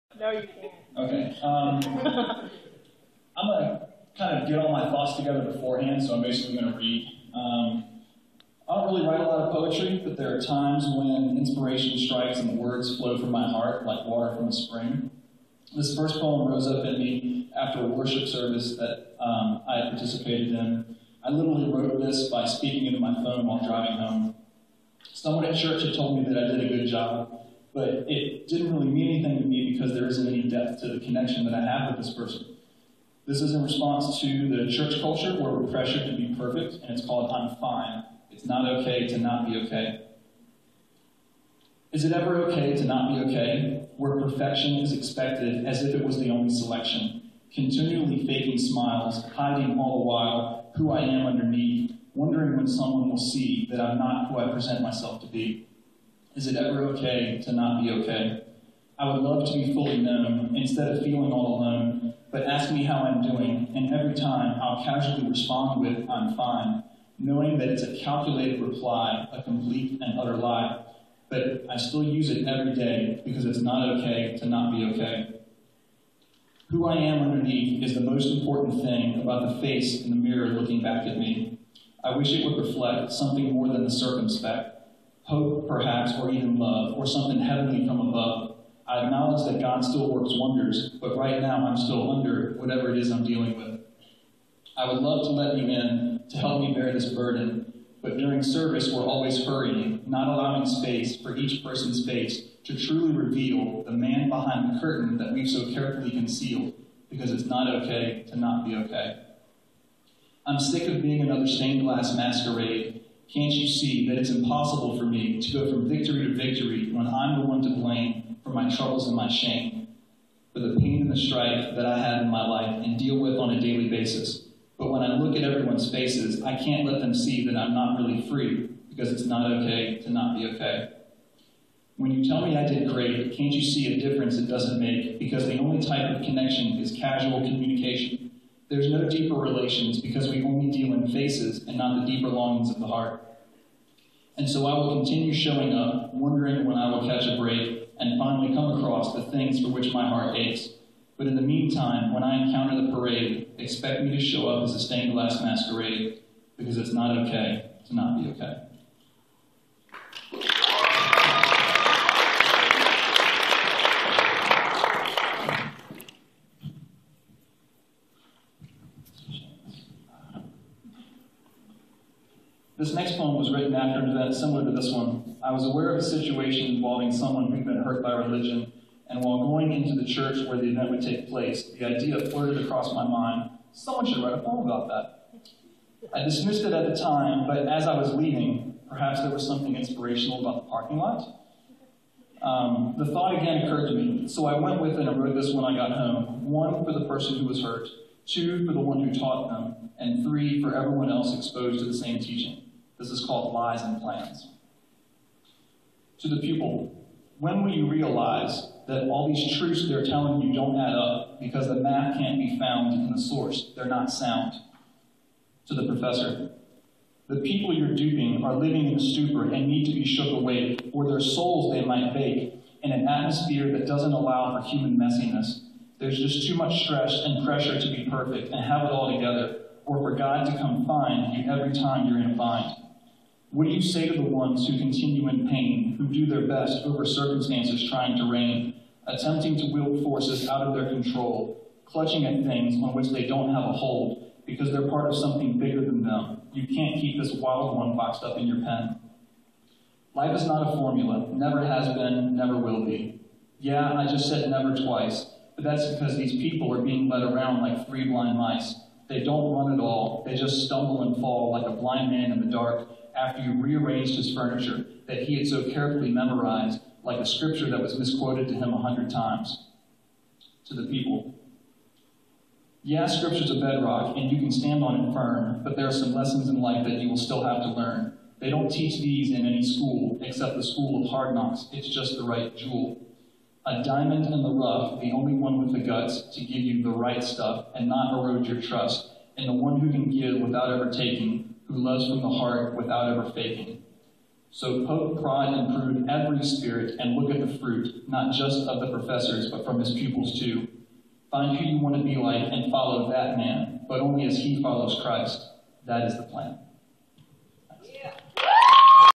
"I'm Fine" & "Lies & Plans" from a YAM Event at Hope Chapel in Amelia, VA
This is me reading some poetry I wrote at an event that Hope Chapel hosted on June 21, 2014. The poems are too long to include in the video description, but they're linked below.

My apologies about the video being turned sideways halfway through; I don't think the person who took it realized that it would be sideways afterward. :)

I don't really write a lot of poetry, but there are times when inspiration strikes and the words flow from my heart like water from a spring. This first poem rose up in me after a worship service that I had participated in (I literally wrote this by speaking into my phone while driving home). Someone at church had told me that I did a good job, but it didn't really mean anything to me because there isn't any depth to the connection that I have with this person. This is in response to the church culture where we're pressured to be perfect, and it's called "I'm fine": It's Not Okay to Not Be Okay (text at link below):

This next poem was written after an event similar to this one. I was aware of a situation involving someone who'd been hurt by religion, and while going into the church where the event would take place, the idea flirted across my mind, "Someone should write a poem about that." I dismissed it at the time, but as I was leaving (perhaps there was something inspirational about the parking lot?), the thought again occurred to me. So I went with it and wrote this when I got home (1) for the person who was hurt, (2) for the one "taught" them, and for (3) everyone else exposed to the same teaching. This is called Lies & Plans (text at link below):